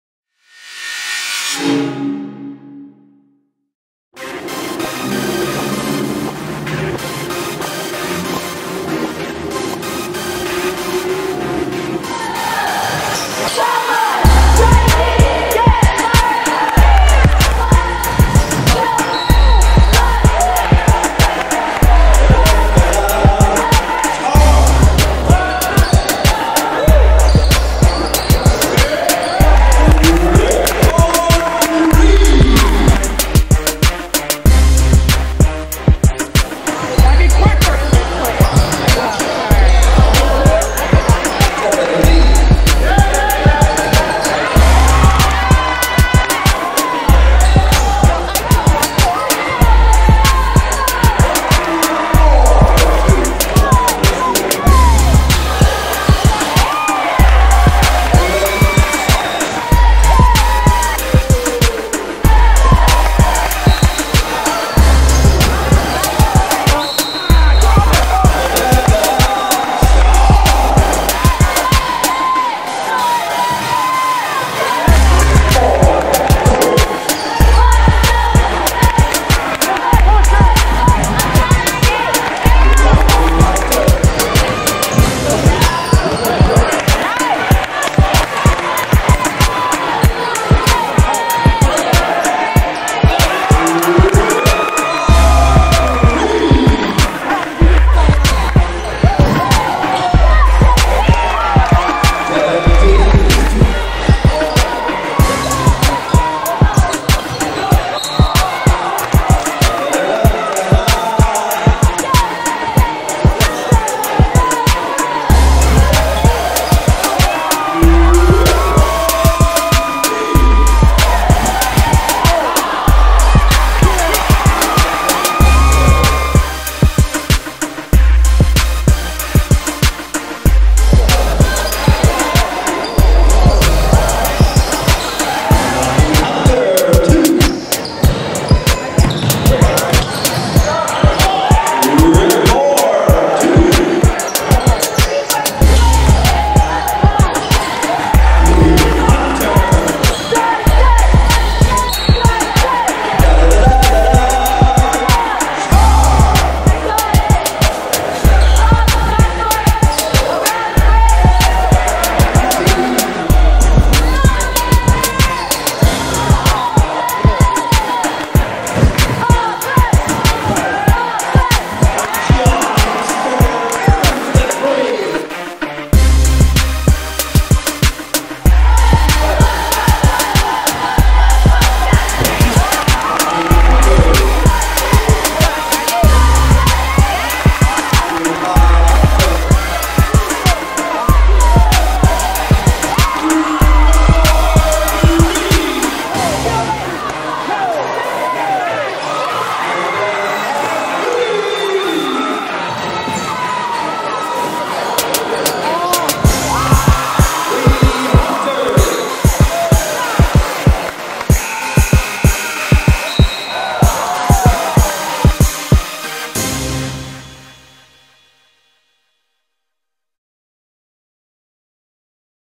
Regional Quarterfinal 2-14-13 Mariner Basketball
Mariner 64  Golden Gate 68
Rodney Hunter led the Tritons with a game-high 27 points. Senior Rick Moore scored 18, including four three-pointers.